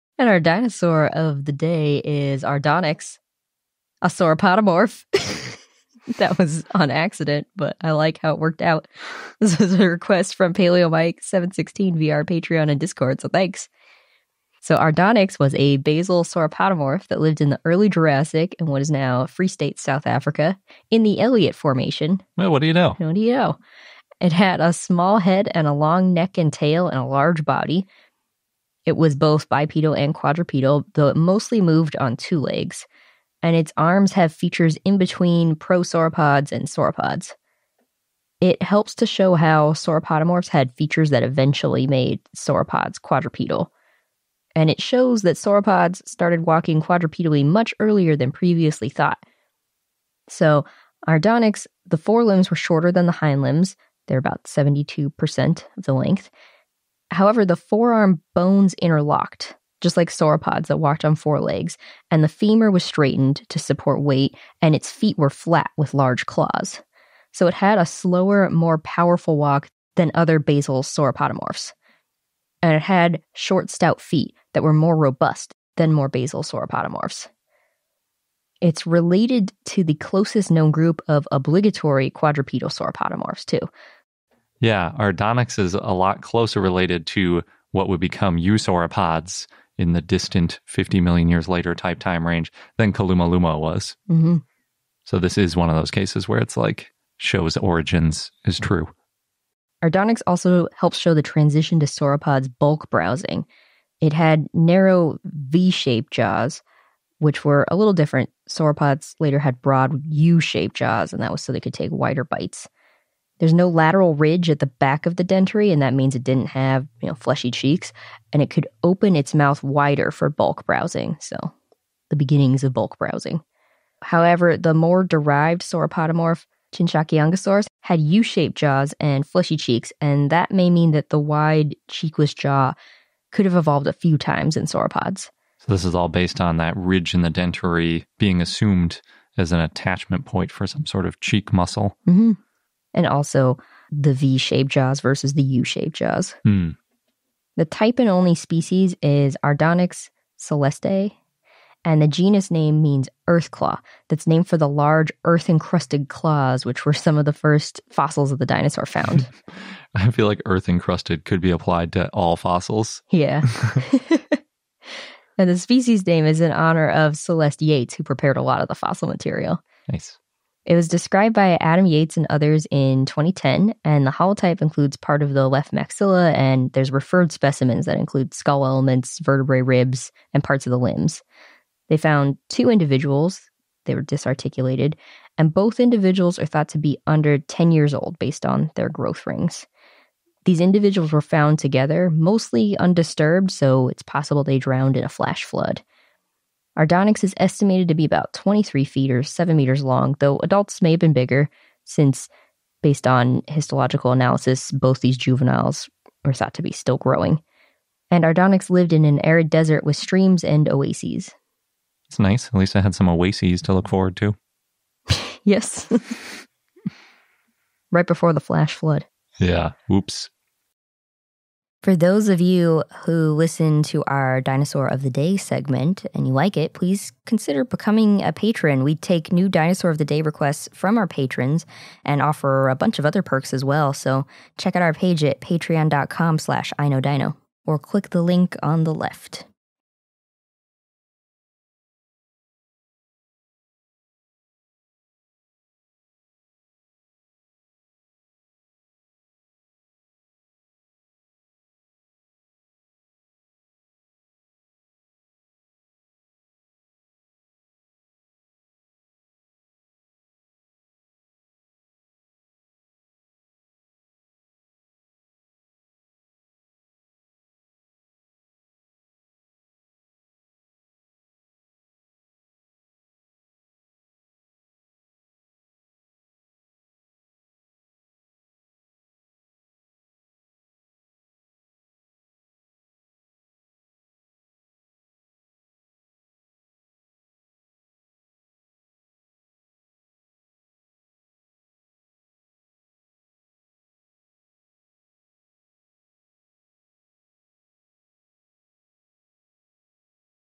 Aardonyx: Dinosaur of the Day
For the full episode where this dinosaur of the day was originally featured check out

Basal sauropodomorph that lived in the Early Jurassic in what is now Free State, South Africa (Elliot Formation)
Had a small head and long neck and tail, and a large body
Both bipedal and quadrupedal, though mostly moved on two legs
Arms have features in between prosauropods and sauropods
Helps show how sauropodomorphs had features that eventually made sauropods quadrupedal
Shows that sauropods started walking quadrupedally much earlier than previously thought
Forelimbs where shorter than hindlimbs (about 72%)
However, forearm bones interlocked, like sauropods that walked on four legs, femur was straightened, to support weight, and feet were flat with large claws
Had a slower, more powerful walk than more basal sauropodomorphs
Had short, stout feet, more robust than more basal sauropodomorphs
Related to the closest known group of obligatory quadrupedal sauropodomorphs
Shows transition to sauropods bulk-browsing
Had narrow, V-shaped jaws (sauropods have broad, U-shaped jaws for wider bites)
No lateral ridge at the back of the dentary, which means didn't have fleshy cheeks, and could open its mouth wider for bulk browsing
However, the more derived sauropodomorph Chinshakiangosaurus had U-shaped jaws and fleshy cheeks, which may mean the wide, cheekless jaw may have evolved a couple times in sauropods